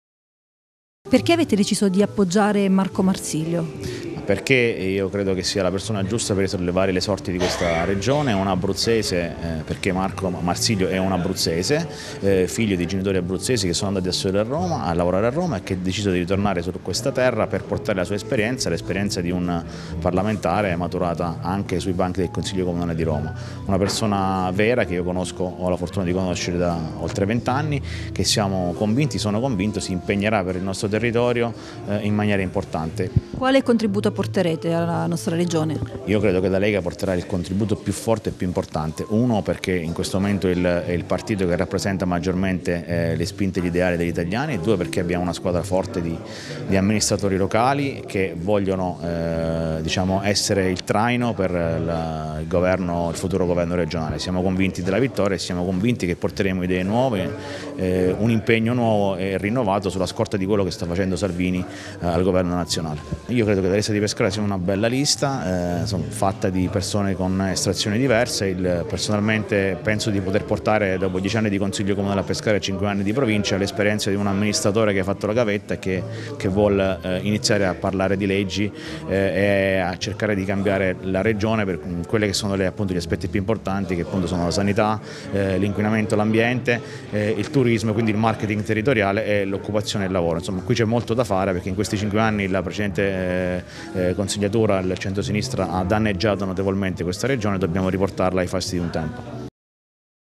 Lega. Open Day per D'Incecco a sostegno di Marsilio
Oggi open day del comitato elettorale del candidato al consiglio regionale per la Lega Vincenzo D'Incecco in corso Vittorio Emanule a Pescara